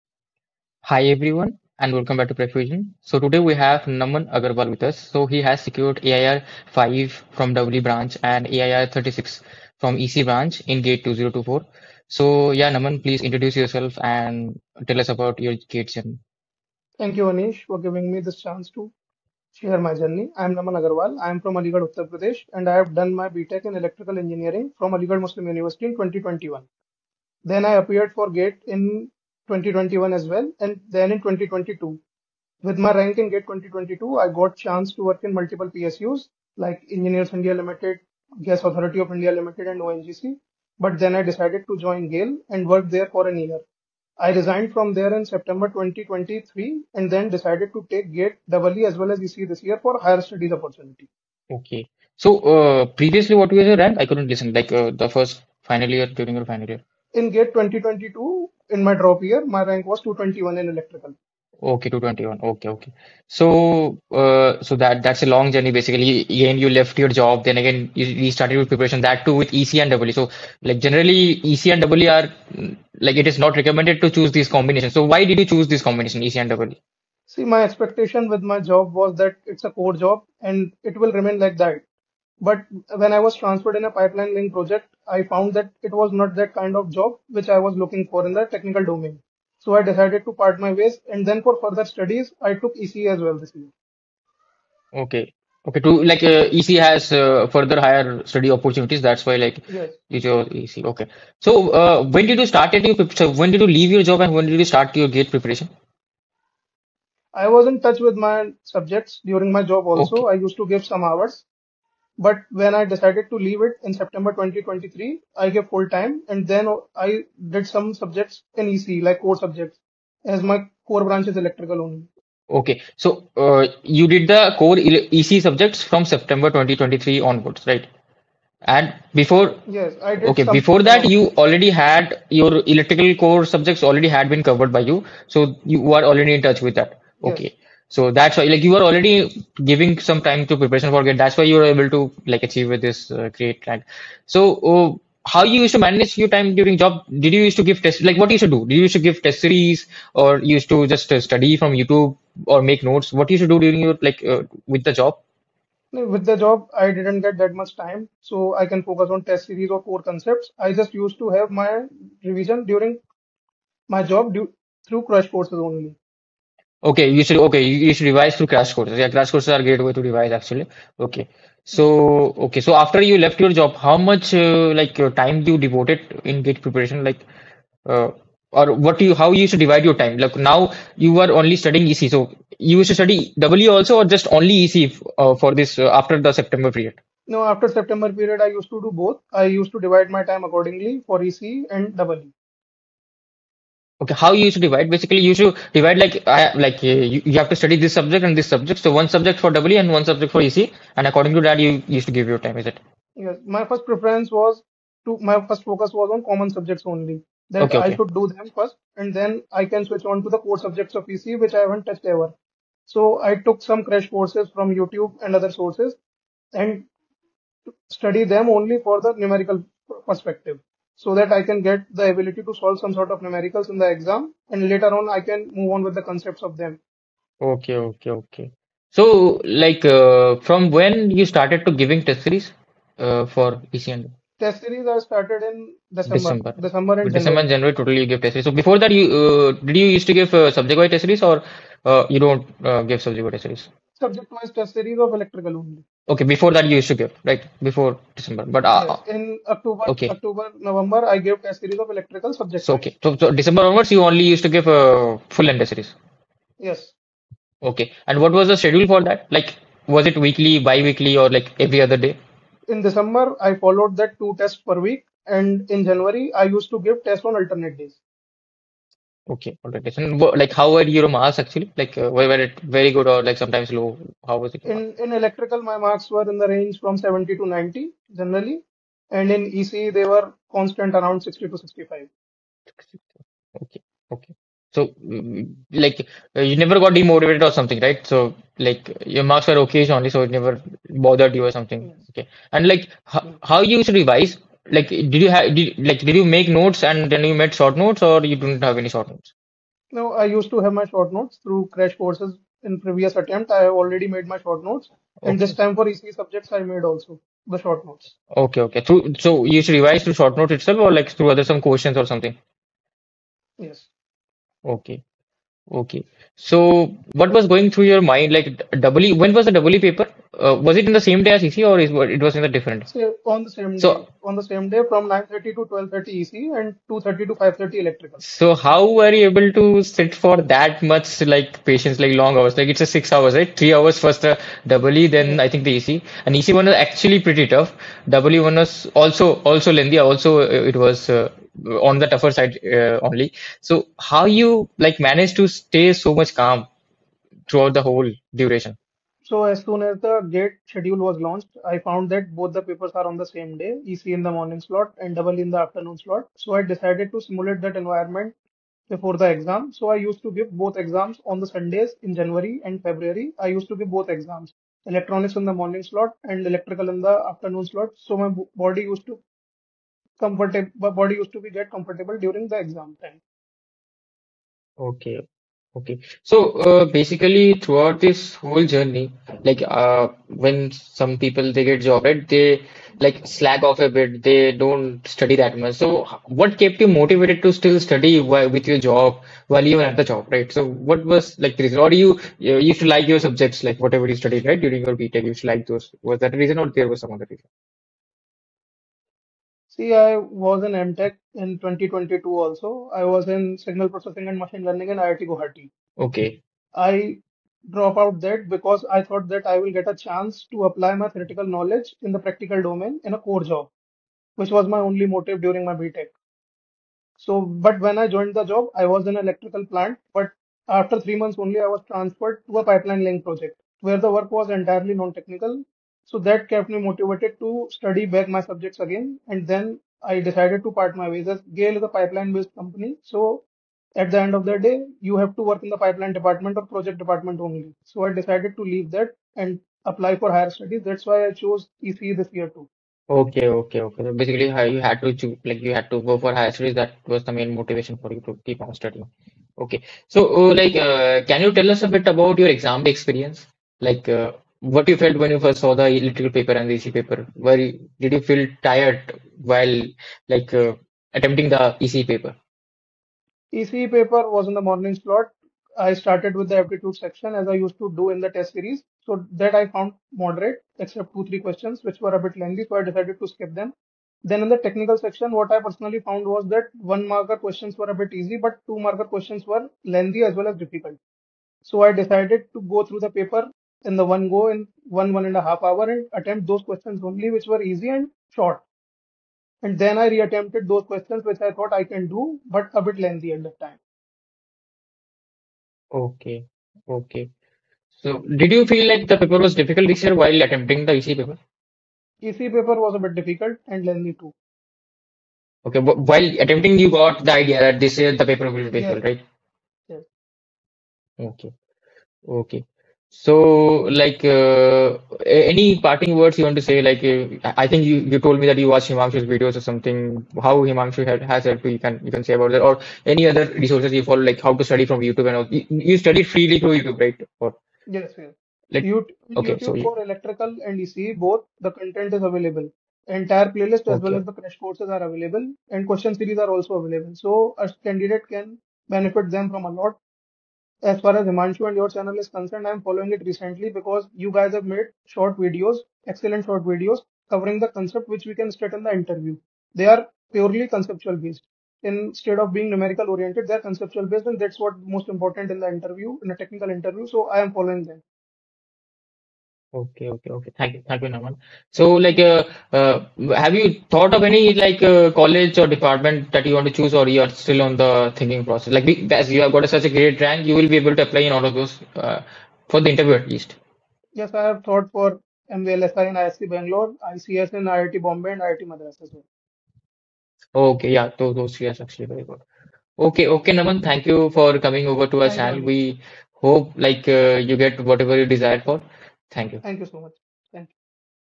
Interview with GATE AIR 5 (EE) & AIR 36 (ECE) || Naman Agarwal || PrepFusion
Meet with our top Ranker, Naman Agarwal, who secured AIR 5 (EE) & AIR 36 (ECE) in the GATE'24 exam.